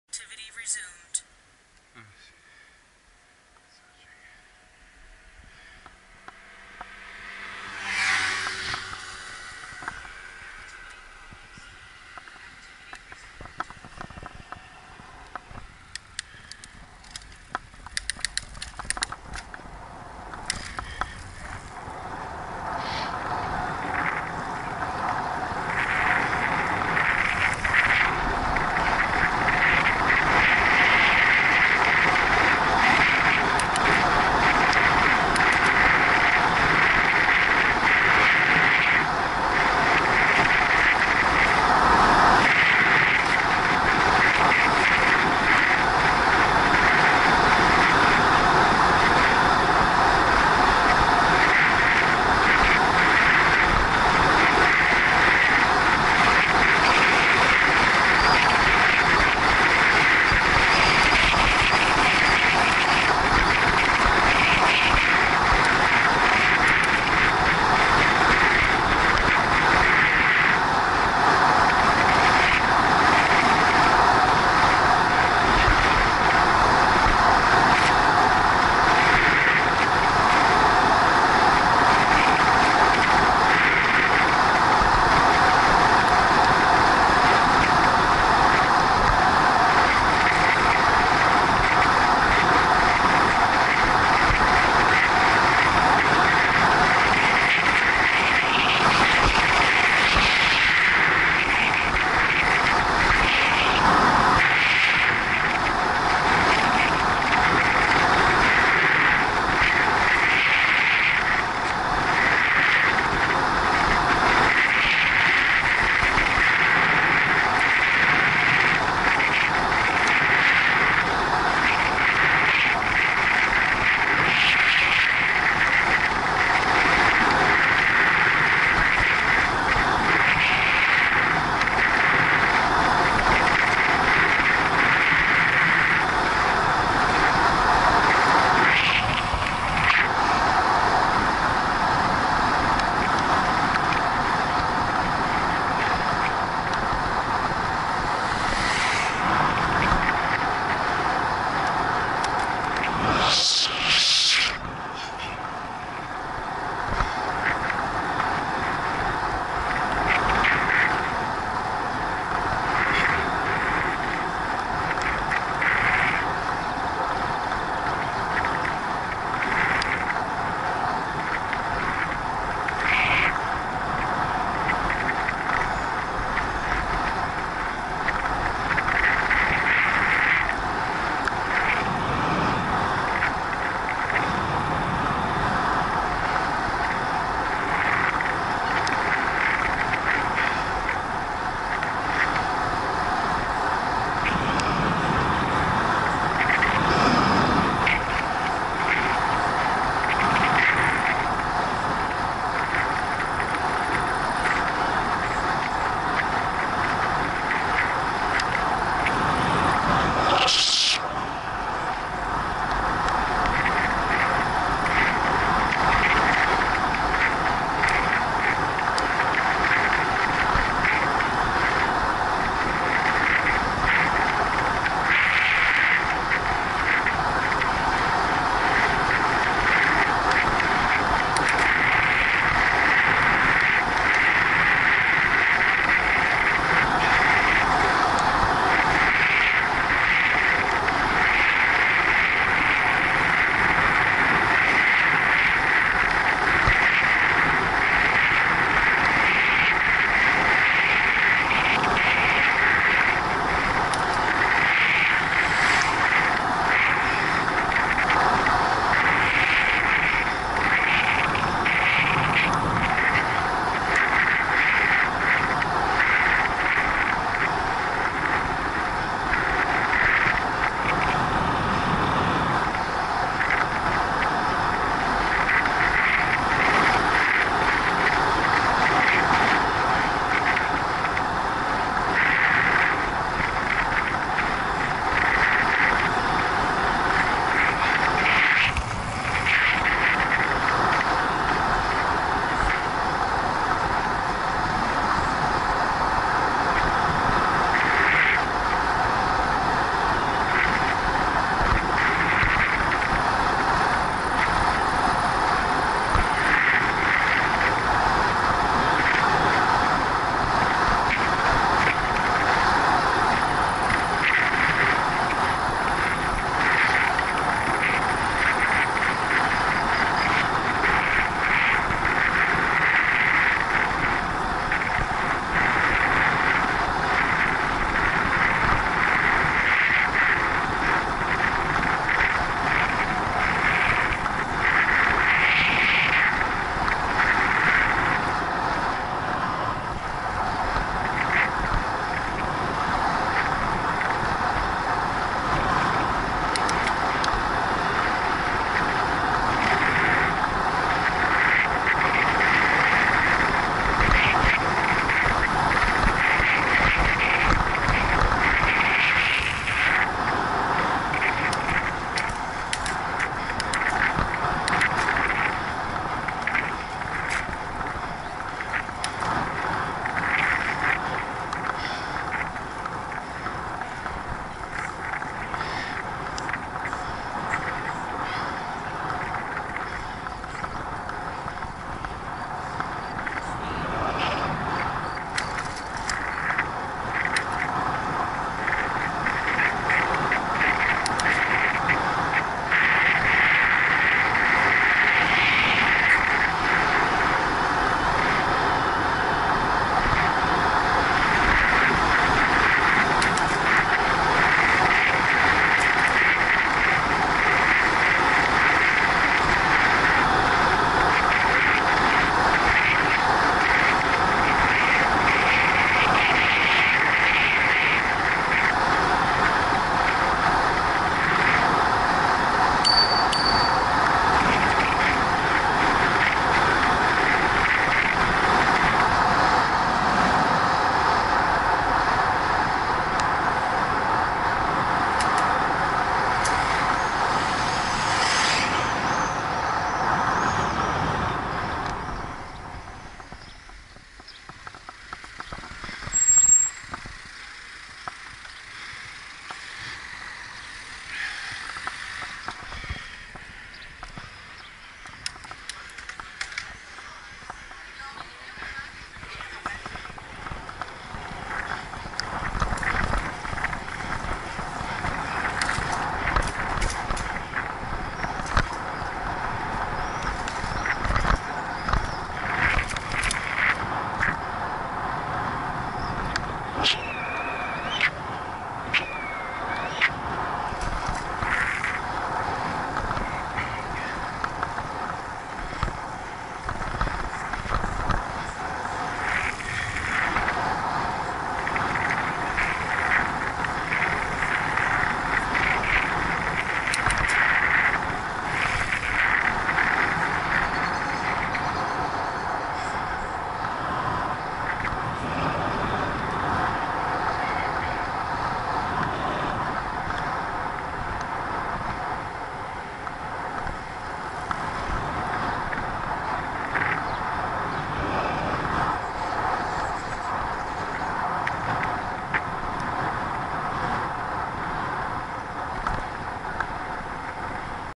Bicycle Combovin - Chabeuil Sony HDR-AS30V GPS overlay
Segment where I go from Combovin to Chbeuil in Rhone-Alpes, France with my bycicle and recording using a Sony HDR-AS30V with GPS data overlay registered by the camera. Started after a bike but there were so many mosquitos that I always had to lower my head or else I would eat them all. A very hot day, about 32º